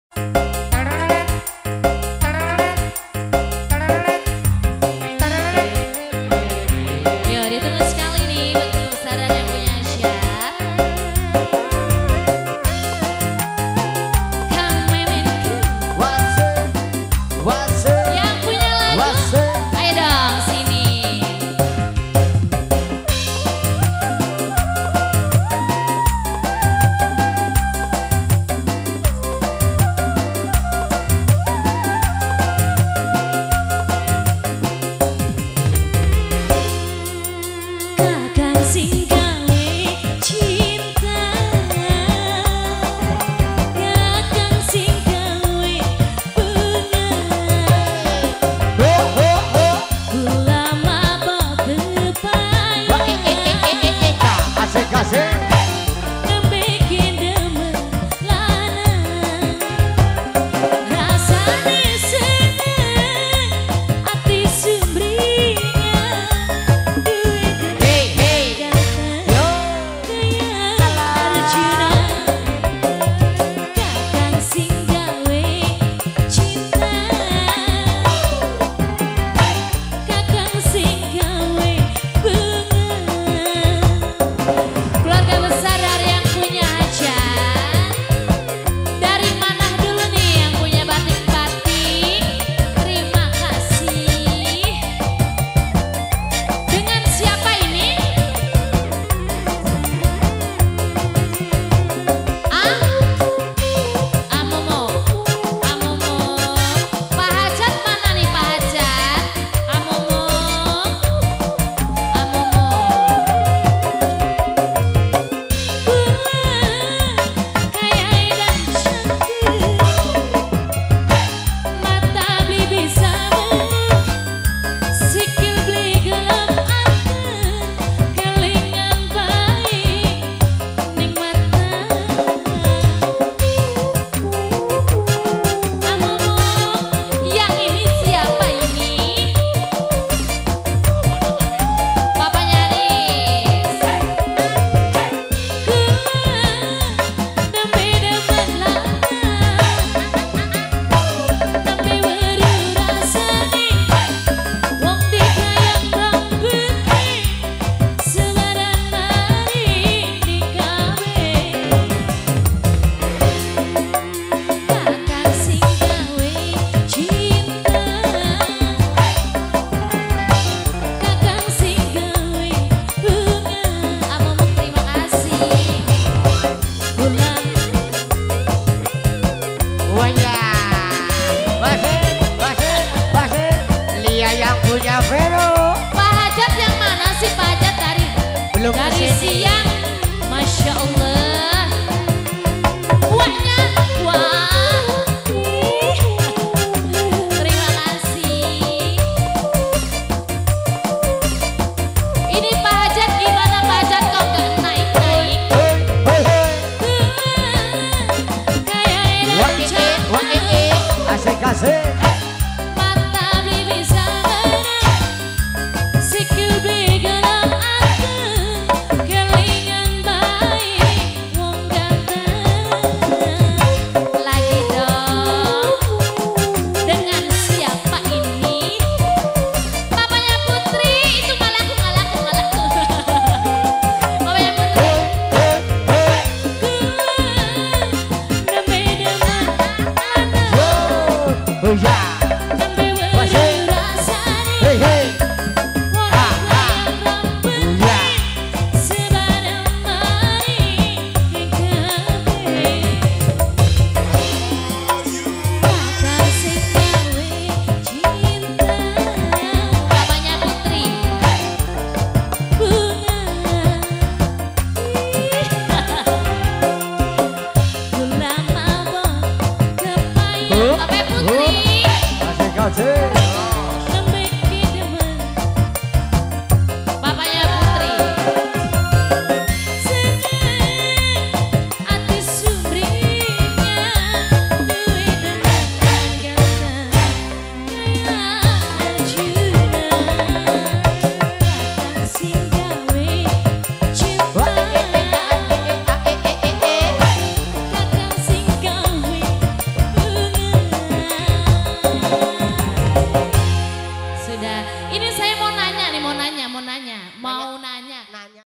18 KANG MEMEN ENTERTAINMENT @ Loji Jatiwangi Majalengka
Adi Karya Loji, 7 Agustus 2019
Khitanan Dede Supandi
Dusun 03 Loji Jatiwangi Majalengka
Keluarga Hajat
Yani (Ang Yani)    Lilis Trisnawati (Eno)

KANG MEMEN ENTERTAINMENT & KUDA RENGGONG GAMBIR ANOM
Pimp. Kang Memen Sindangwasa